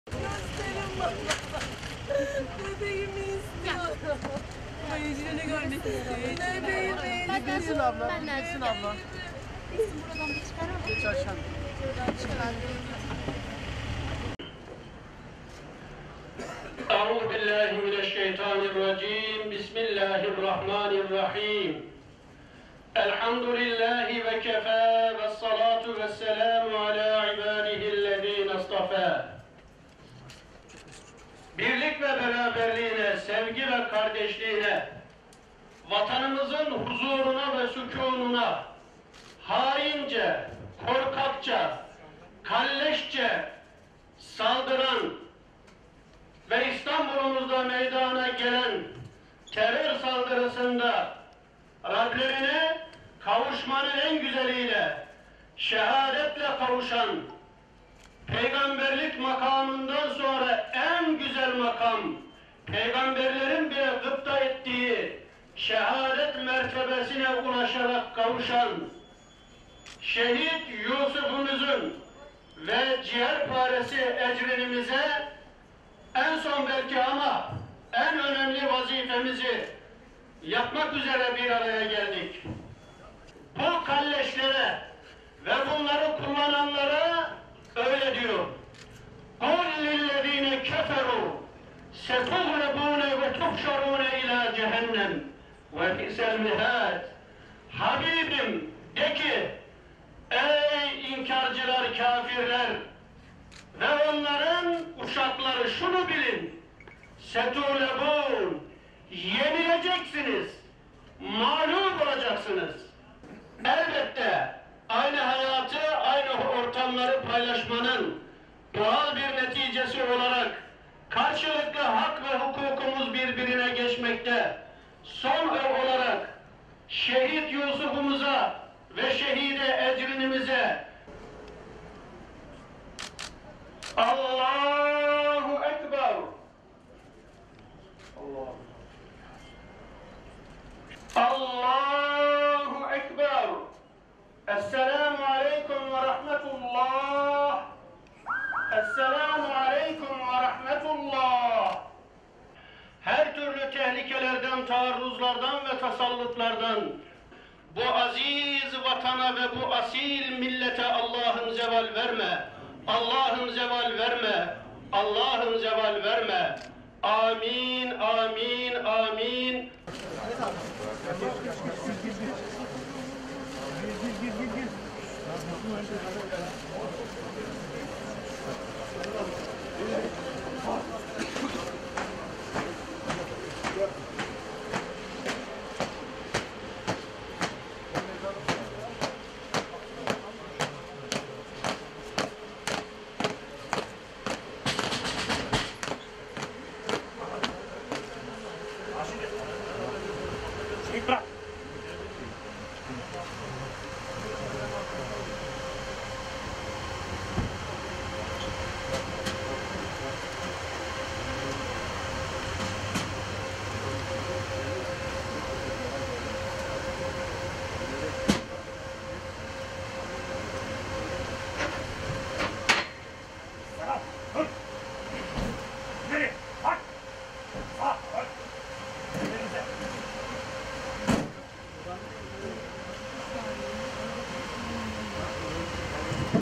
Minik Ecrin’in Annesinden Yürek Yakan Haykırış: Gösterin Bana, Bebeğimi İstiyorum! - TGRT Haber
Beyoğlu İstiklal Caddesi'nde 6 kişinin yaşamını yitirdiği, 81 kişinin ise  yaralandığı bombalı terör saldırısında hayatlarını kaybeden baba ve kız Yusuf ve Ecrin Meydan'ın yakınları gözyaşlarına boğuldu. Patlamadan yaralı olarak kurtulan Mevlüdiye Meydan ise, kızı ve eşinin fotoğraflarını öperek, "Üzerimde kocamla kızımın kanı var. Gösterin bana, bebeğimi istiyorum. Doyamadım ben kızıma.” dedi.